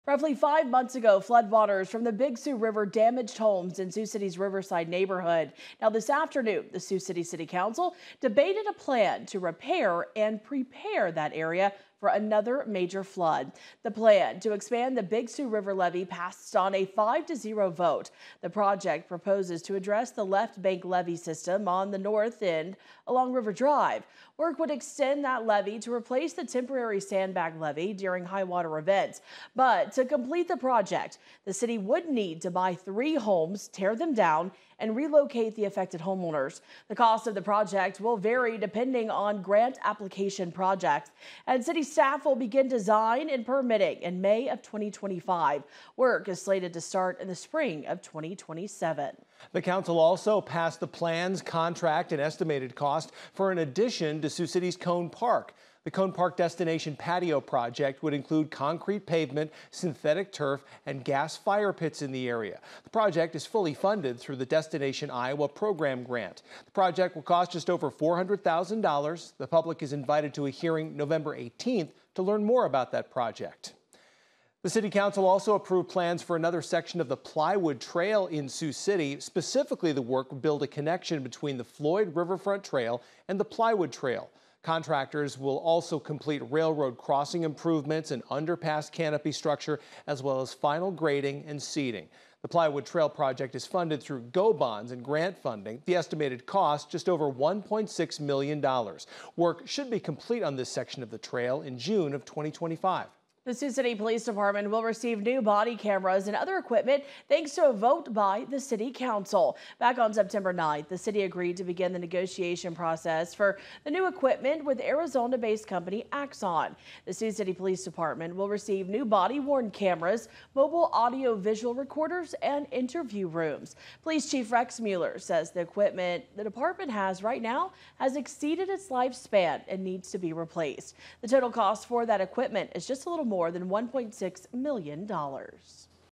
Sioux City City Council approves Big Sioux Levee Project, and new equipment for Sioux City Police...
Sioux City City Council approves Big Sioux Levee Project, and new equipment for Sioux City Police Department